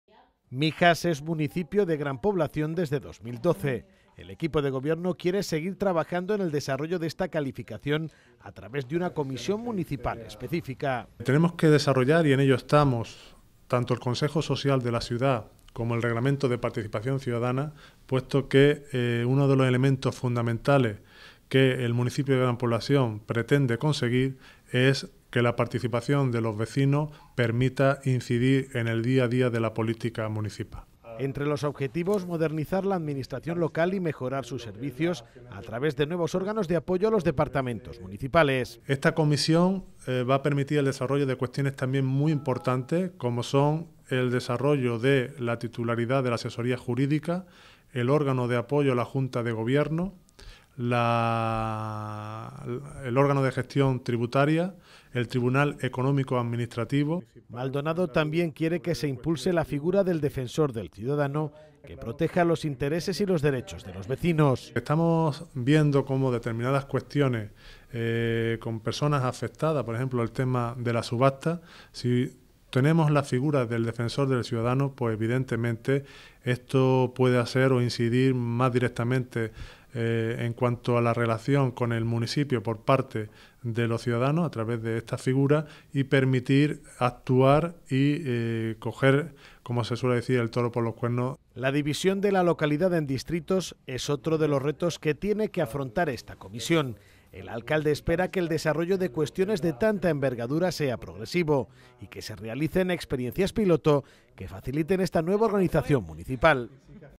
Municipio de Gran Población
El Ayuntamiento da luz verde a la comisión para el desarrollo de Mijas como Municipio de Gran Población